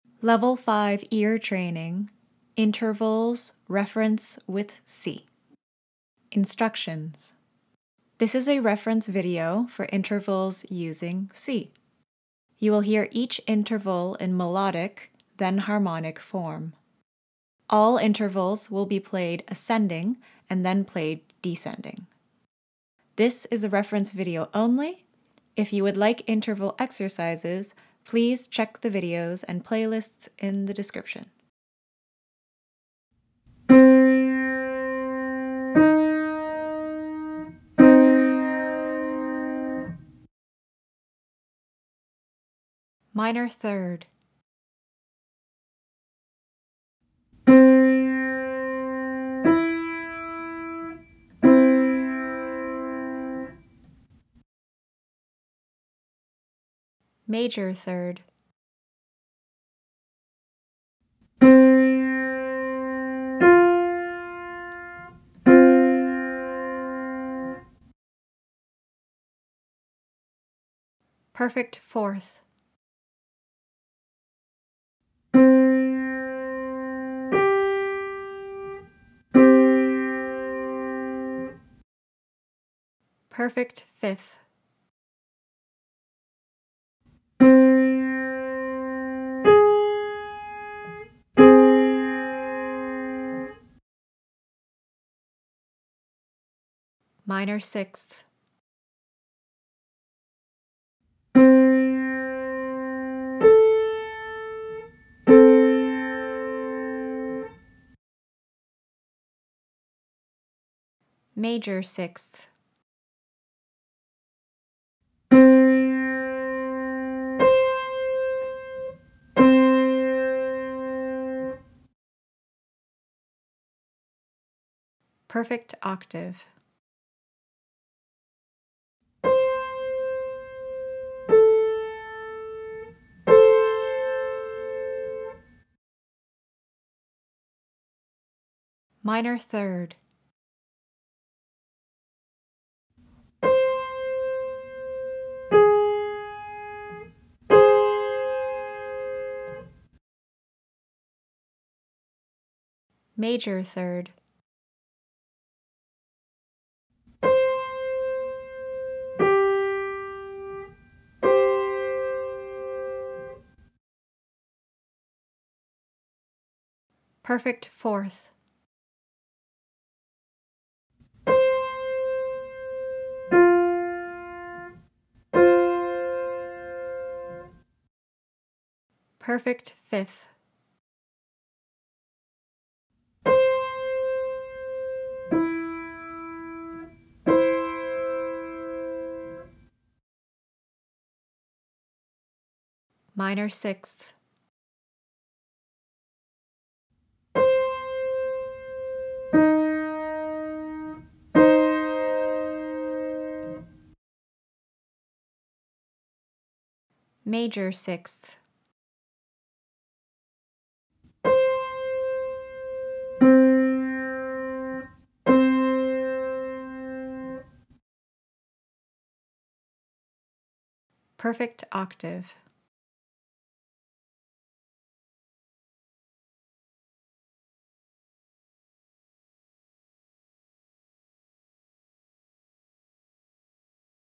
RCM Level 5 Intervals Reference with C (Ear Training -3, +3, p4, p5, -6, +6, p8 Identification)
Interval Identification References on C (Played on Piano)
(Musicianship / Ear Tests / Intervals)
Timestamps:

• (0:00) Instructions
• (0:32) Ascending from C
• (2:08) Descending from C

Interval options: minor third, major third, perfect fourth, perfect fifth, minor sixth, major sixth, perfect octave

The exercises in this video were created based on the requirements from the RCM Piano Syllabus (2022 Edition and 2015 Edition). The RCM Violin Syllabus (2021 Edition) has similar requirements.